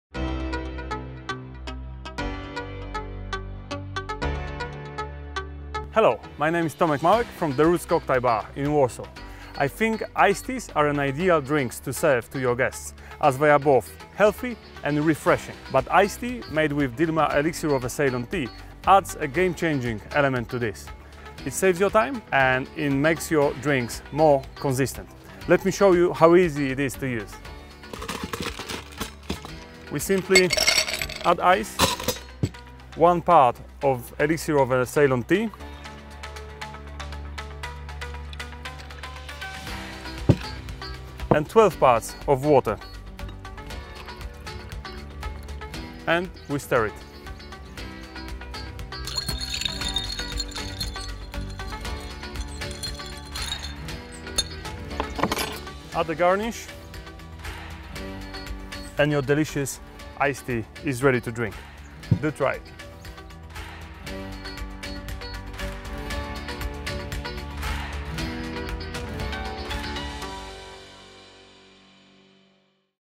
Real Dilmah Elixir Iced Tea
Dilmah Real Elixir Iced Tea can be prepared in 5 steps, watch World Champion Flair Bartender show you how.